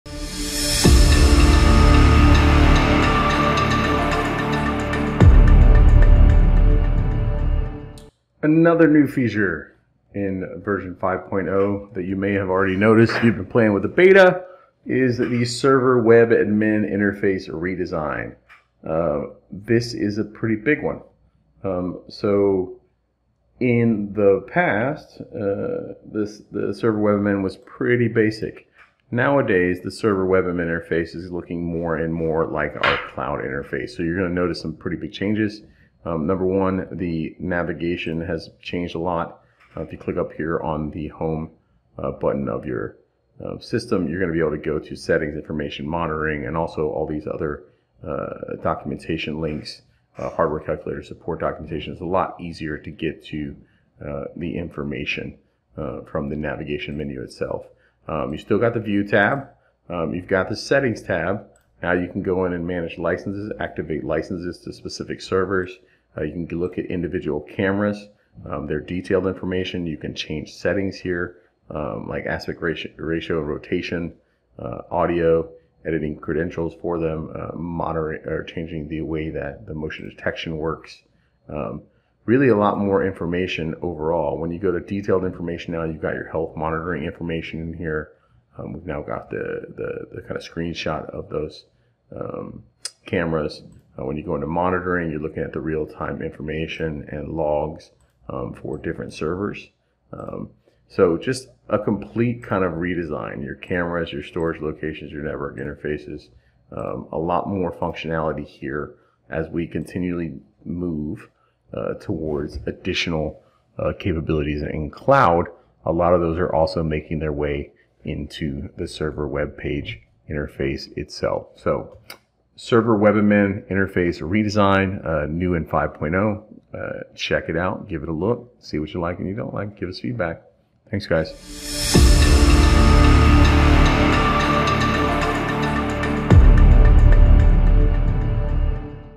Server Web Admin Redesign - Nx Witness v5
New in Nx Witness VMS v5.0 - The Server Web Admin interface has been redesigned to allow administrators to add and remove users, as well as view detailed information about Servers, Cameras, and Licenses.